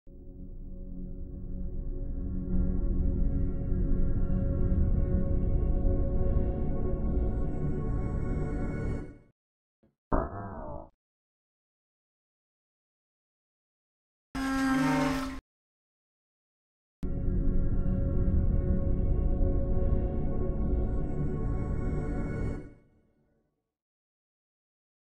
Poulet crudites
Exercice d'animation en 1 journée avec décor et sons imposés, réalisé à l'EMCA : Ecole des métiers du cinéma d' Animation (en France) - Atelier papier découpé

Merci à Fanny Sonnet et Chloé Ribet !

Animation exercise in 1 day with background and imposed sounds, made at the Emca : Animation Film School (in France) - Paper Cut workshop

Thank you Fanny Sonnet and Chloé Ribet !